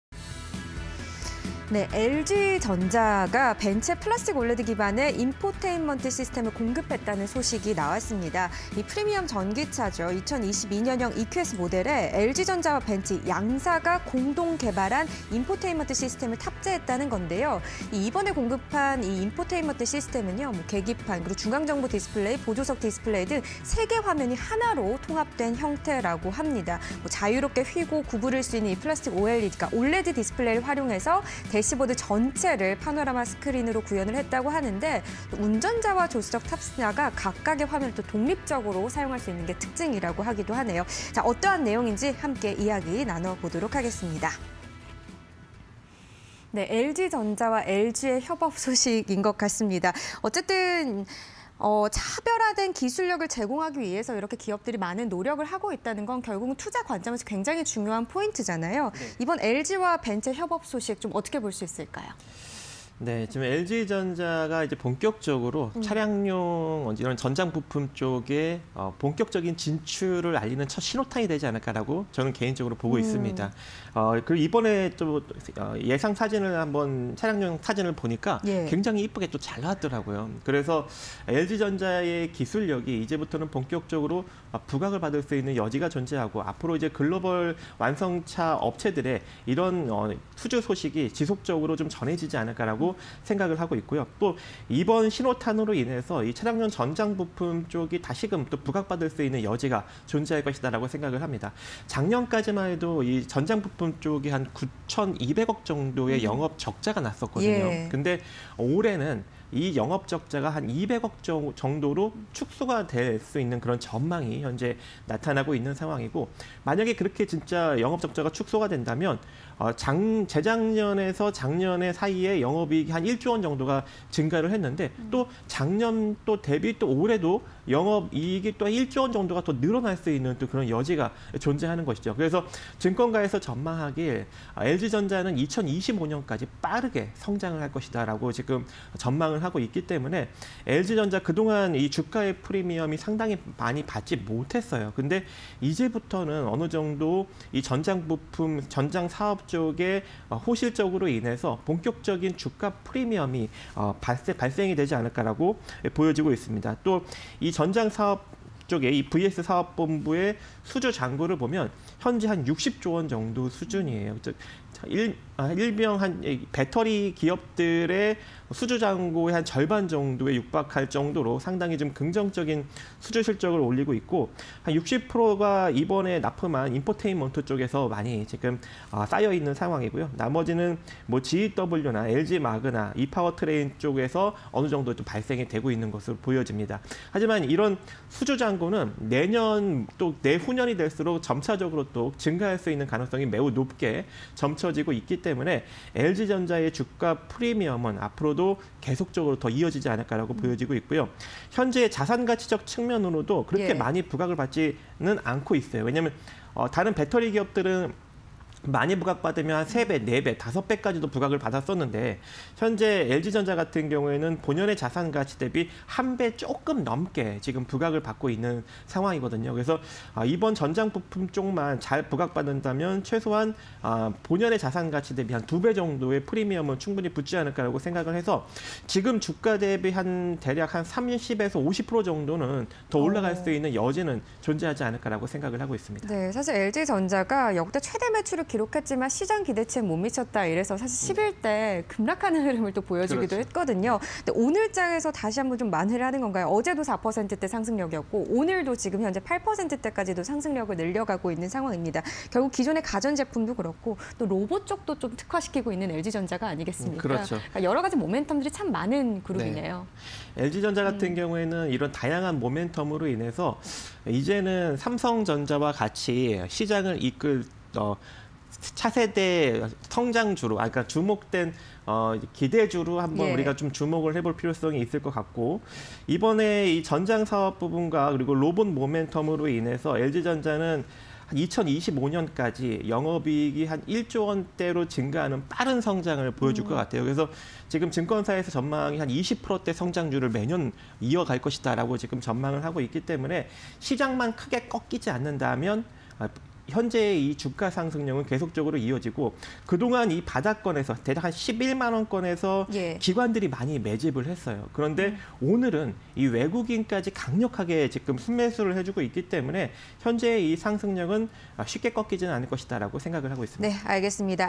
LG전자(066570)... 벤츠에 '올레드' 기반 '인포테인먼트 시스템' 공급_이슈 팩토리 (20220113)
LG전자(066570)... 벤츠 전기차 협력 기대감 확대
- 벤츠에 '플라스틱 올레드' 기반 '인포테인먼트 시스템' 공급
- 계기판·중앙정보디스플레이·보조석디스플레이 등 통합 형태
- 파노라마 스크린으로 대시보드 구현
- 운전자와 탑승자 각각 독립적 화면 사용 가능
- 올해 전장사업 부문, 영업 적자 대폭 감소 전망
- VS 사업본부, 글로벌 탑티어 목표